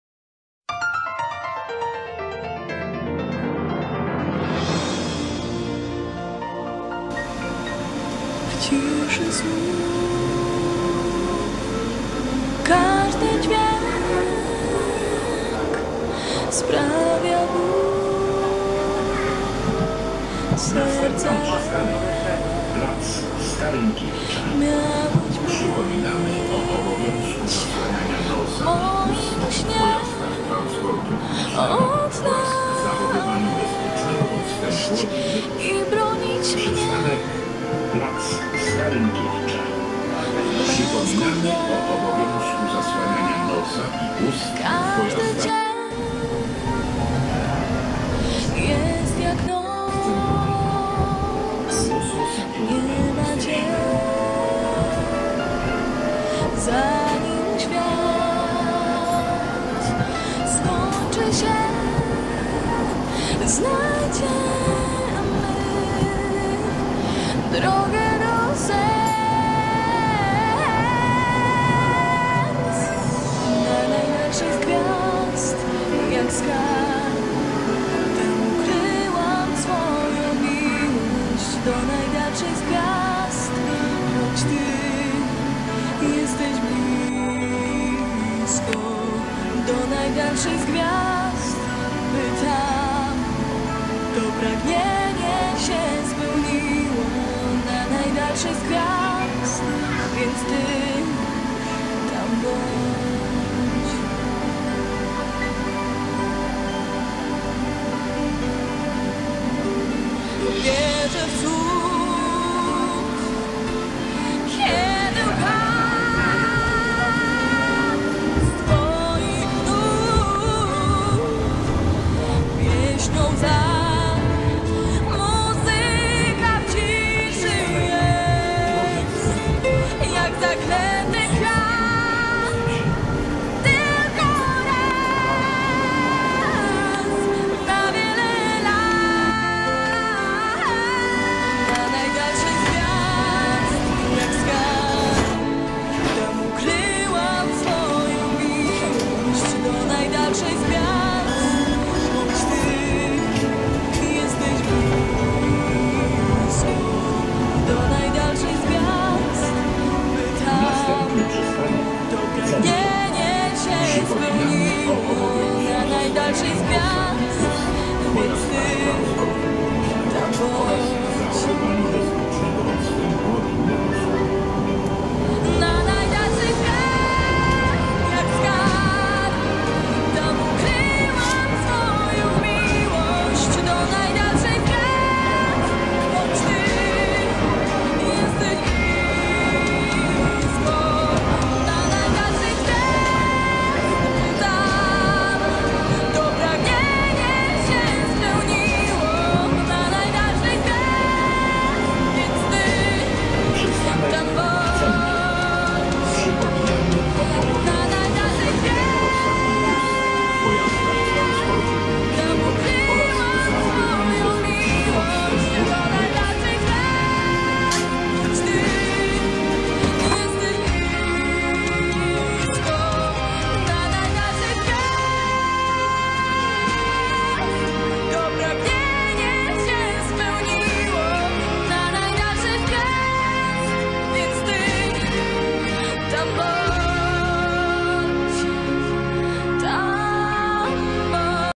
2020 07 19 Warszawa Aleje Jerozolimskie cz 1 Varso Place Varso Tower w budowie widok z tramwaju po 1
Aleje Jerozolimskie / Jerusalem Avenues / Avenues de Jérusalem
część / part / partie 1
Varso Place
Varso Tower w budowie / under construction / en construction
"Chmielna 89" w budowie / under construction / en construction
widok z tramwaju / view from the tram / vue depuis le tram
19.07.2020 po / after / après 10.58